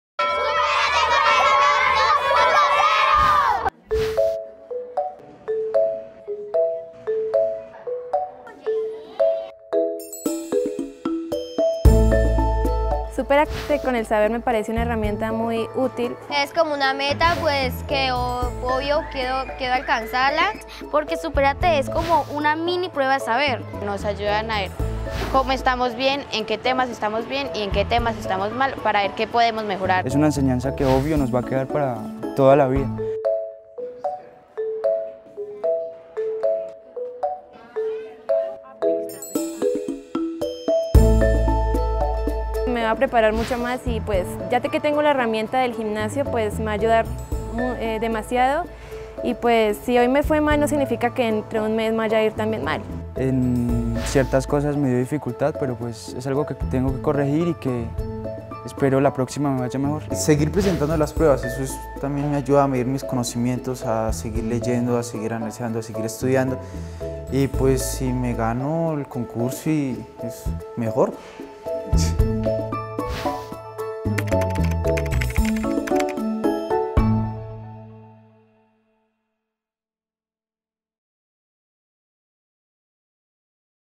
¿Qué es "Supérate con el Saber 2.0" y cómo lograr superarte?
Con esta competencia,  estudiantes de 3°, 5°, 7°, 9° y 11° de todo el país están fortaleciendo sus competencias en lenguaje y matemáticas y, con el acompañamiento de sus docentes, se están preparando para convertirse en los más pilos de Colombia.